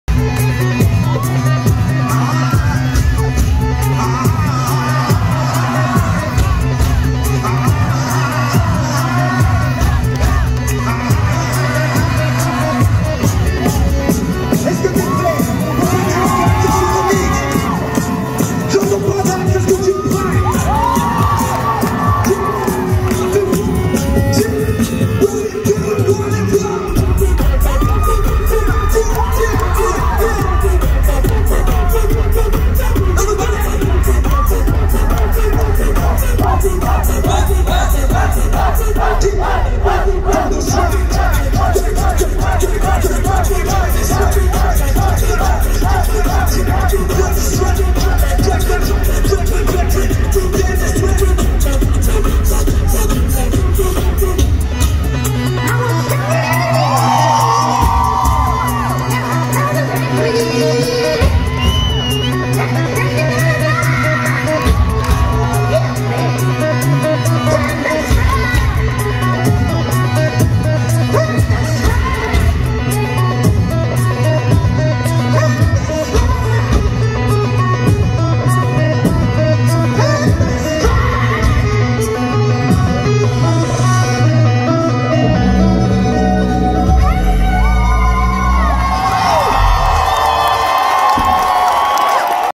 Berywam concert 2022
Arènes de Dax première partie Big Flo et Oli